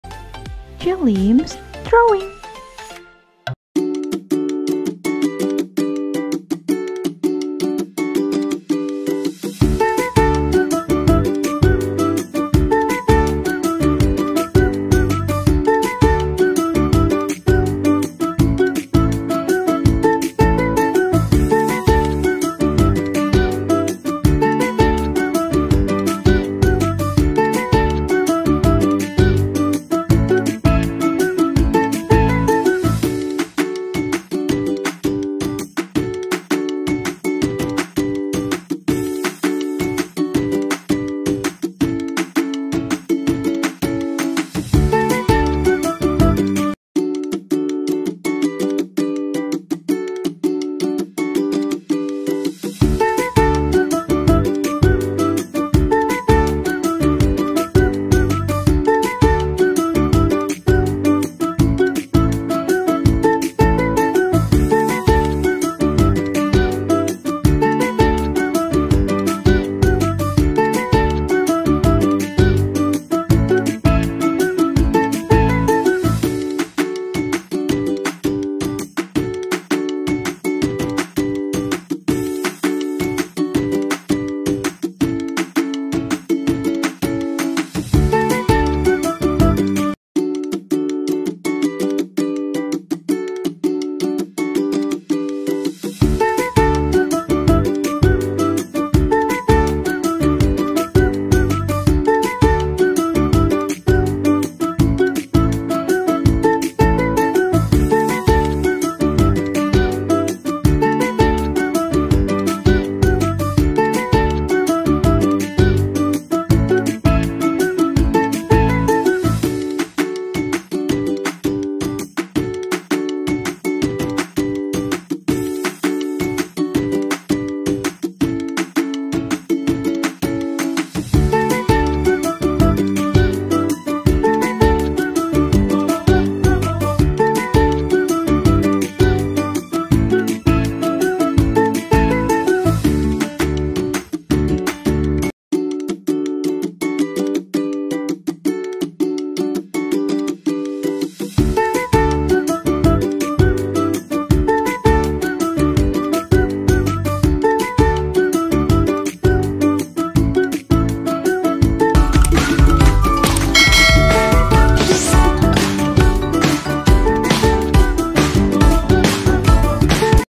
Easy Drawing a Cute Light Bulb for Kids
Hello Guys, welcome to JELIMS DRAWING , in this video you will learn how to draw very Cute Light Bulb
This Light Bulb is very cute and easy to draw, let's draw it together.

~Jelims~

Background Music for Videos
Kids Background Music